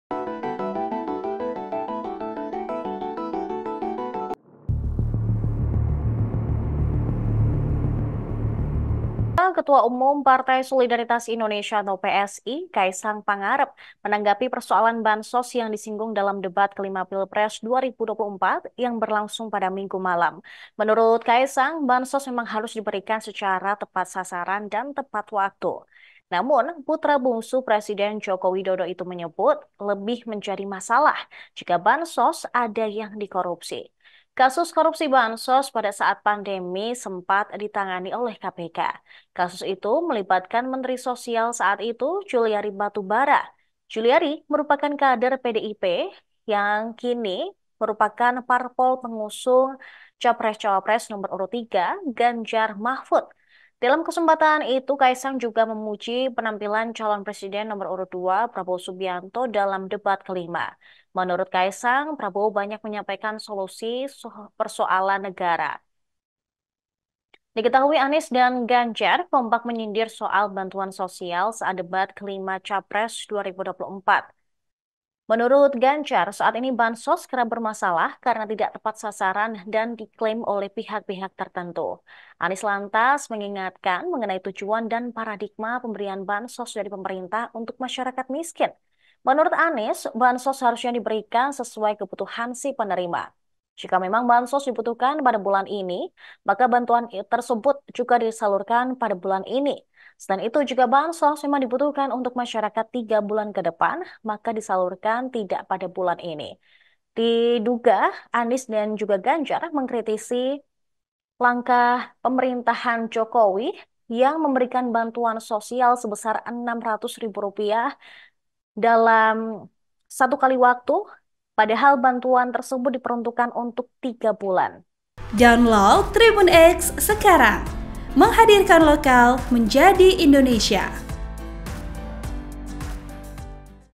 Diduga Sindir PDIP soal Bansos, Kaesang: Jauh Lebih Bermasalah Bansos Dikorupsi saat Pandemi
TRIBUN-VIDEO.COM - Ketua Umum Partai Solidaritas Indonesia (PSI) Kaesang Pangarep menanggapi persoalan bansos yang disinggung dalam debat kelima Pilpres 2024 yang berlangsung Minggu (4/2/2024).

Menurut Kaesang, bansos memang harus diberikan secara tepat sasaran dan tepat waktu.

Namun, putra bungsu Presiden Joko Widodo itu menyebut lebih menjadi masalah jika bansos ada yang dikorupsi.

"Mungkin saat ini dipermasalahkan. Tapi menurut saya jauh lebih bermasalah jika bansos itu saat covid dikorupsi," kata Kaesang di JCC Senayan, Jakarta Pusat, Minggu (4/2/2024) malam.

Kasus korupsi bansos pada saat pandemi sempat ditangani oleh KPK.

Kasus korupsi itu melibatkan menteri sosial saat itu, Juliari Batubara.

Juliari merupakan kader PDI-P, parpol yang kini mengusung Ganjar-Mahfud.

Dalam kesempatan itu, Kaesang pun memuji penampilan calon presiden (capres) nomor urut 2, Prabowo Subianto dalam debat kelima.

Menurut Kaesang, Prabowo banyak menyampaikan solusi persoalan bangsa.

Anies Baswedan dan Ganjar Pranowo kompak menyindir soal bantuan sosial (bansos) saat debat kelima Capres 2024 yang diselenggarakan KPU Minggu malam, 4 Februari 2024.

Menurut Ganjar, saat ini bansos kerap bermasalah karena tidak tepat sasaran dan diklaim oleh pihak-pihak tertentu.

Sumber: Kompas.com
Video Production : Sirajuddin Abbas
Voice Over: Sara